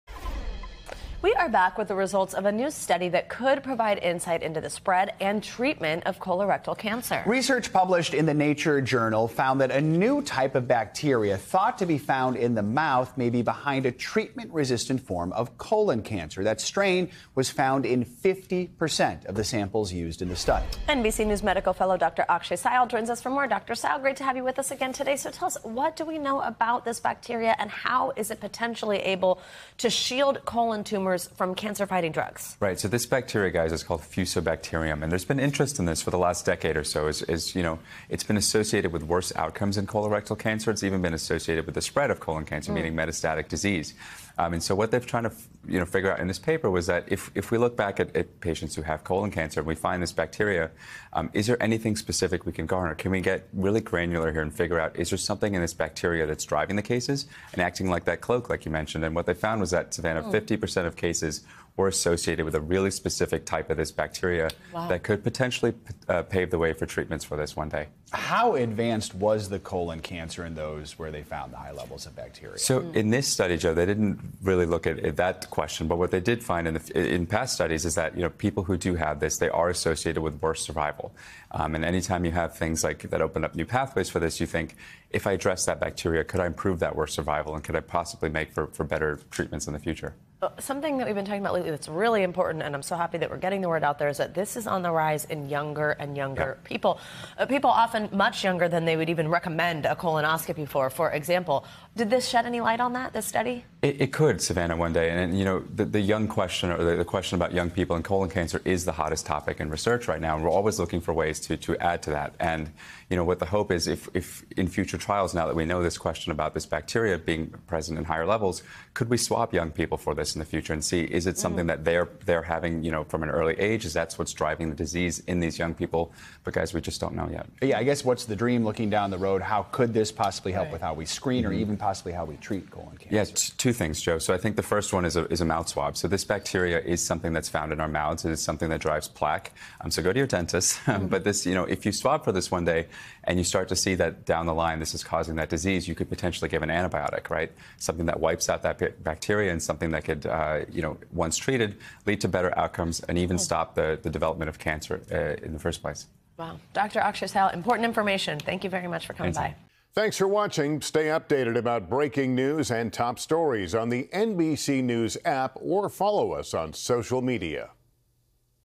Study finds a new type of bacteria present in colorectal cancer
According to a study in the scientific journal Nature, a new type of bacteria found in the mouth may be behind a treatment-resistant form of colon cancer. NBC News’ Dr. Akshay Syal explains how this bacteria is potentially able to shield colon tumors from cancer-fighting drugs.